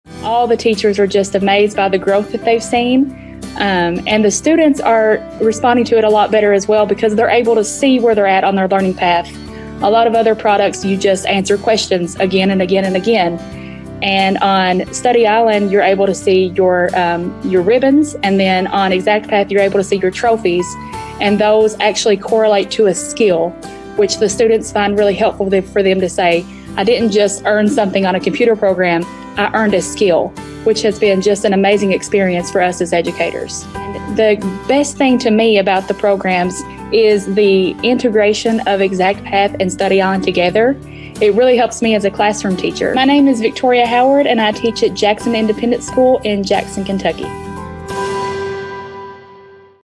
Growth and Proficiency with Study Island and Exact Path
Victoria Howard, Jackson ISD, KY talks about the success she has seen with her students in the classroom using Study Island and Exact Path together.